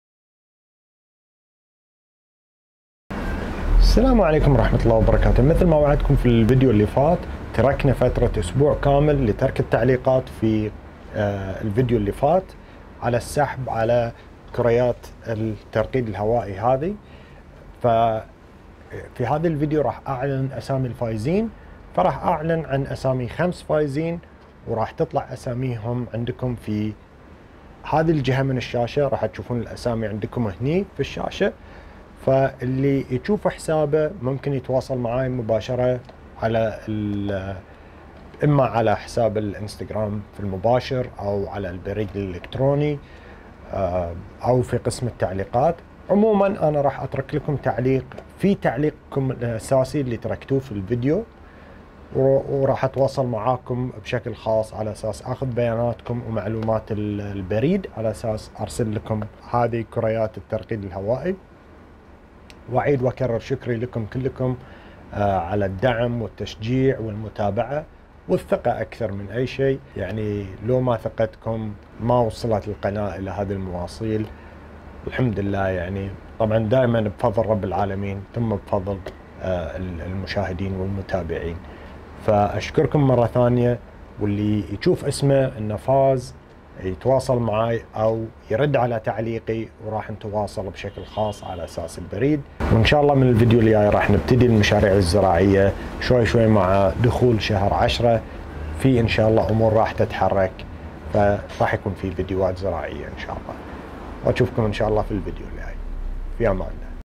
إعلان الفائزين بالسحب على كور الترقيد الهوائي
أولا أود شكر جميع المتابعين على دعمهم و تشجيعهم و تفاعلهم
الفائزين بالسحب هم:
1) سبيل الرشاد
2) Mohamed Abouzour
3) April I
4) ابوالزبير اللنجاوي
5) سلطان العليان

أو على الخاص في انستقرام
grow_gardening

مبروك للفائزين